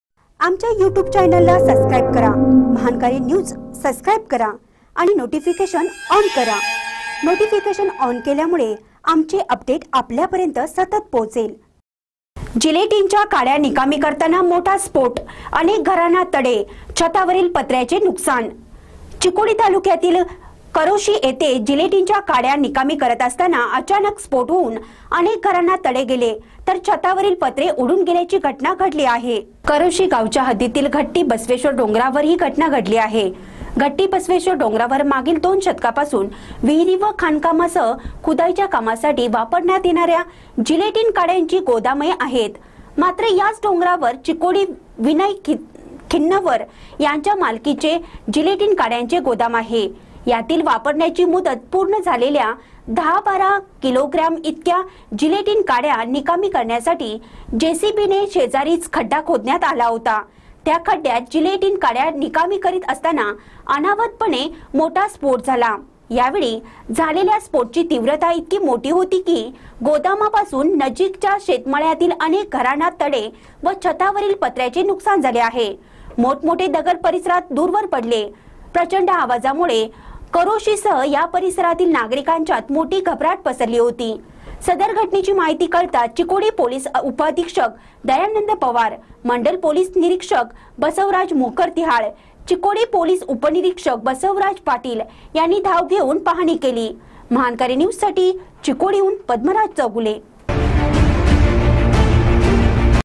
जिलेटीनच्या कांड्या निकामी करताना मोठा स्फोट
जिलेटीनच्या कांड्या निकामी करताना मोठा स्फोट : अनेक घरांना तडे, छतावरील पत्र्यांचे नुकसान